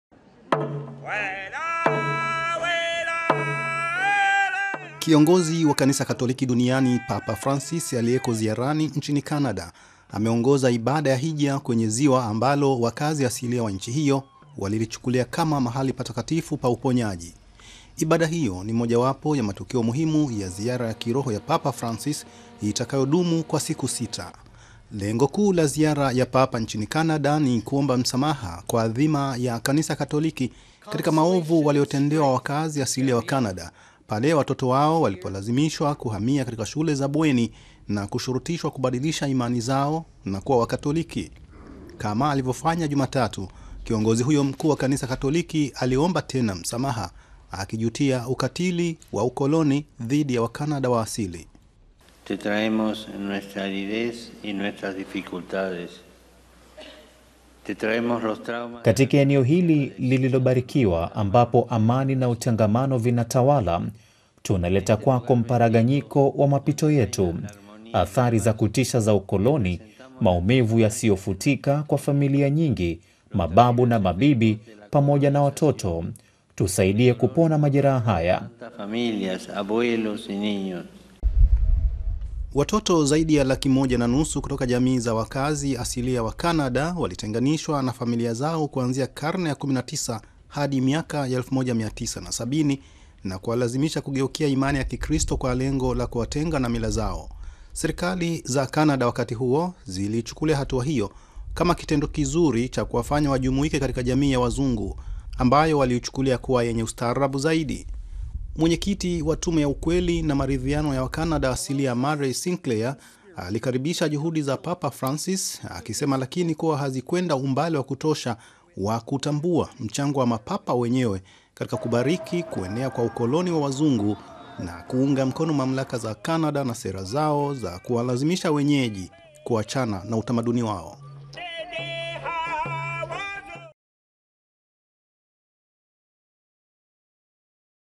Papa Francis aendelea na zaiara ya kutubu Canada
Kiongozi wa Kanisa Katoliki duniani Papa Francis ameendelea kuomba msamaha kwa mchango wa Kanisa hilo katika maovu waliyofanyiwa wakaazi asilia wa Canada, ambao walishurutishwa kuacha mila zao na kugeukia ukristu na tamaduni za Wazungu.